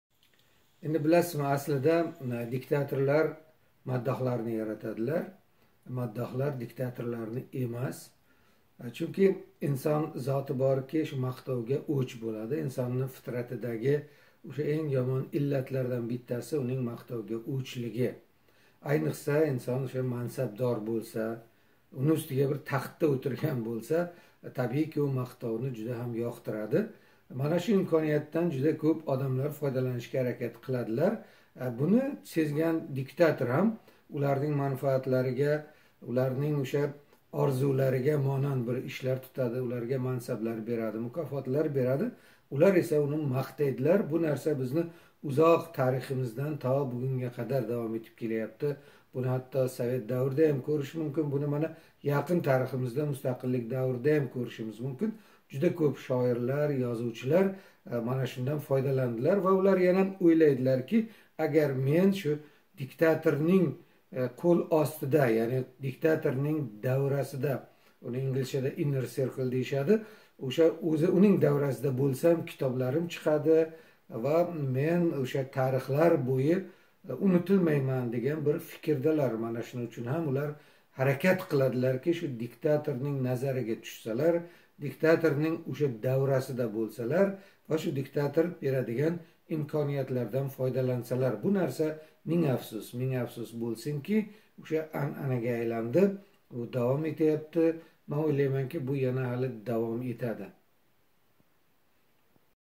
Jahongir Mamatov: BBC savollariga javoblar-1. (2017)
1.O'zbekistonda boshlangan yangicha maddohlik  ikkinchi diktatorni yaratib qo'yishi xavfi bormi?
 Bunga yo'l qo'ymaslik uchun nima qilish kerak va hozirgi baxshida  she'r yozishlarga qanday qaraysiz?